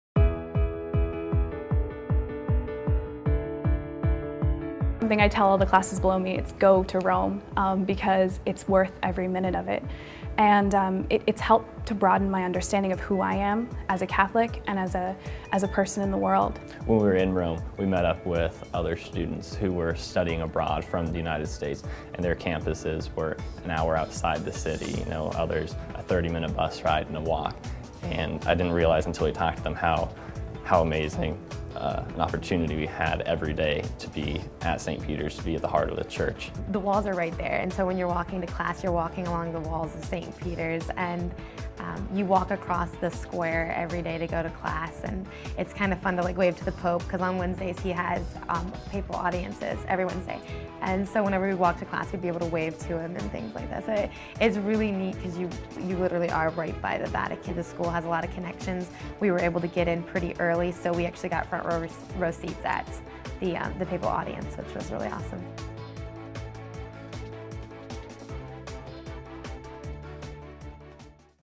A Global Experience | Junior Semester Rome
This video is about Christendom College's Junior Semester in Rome Program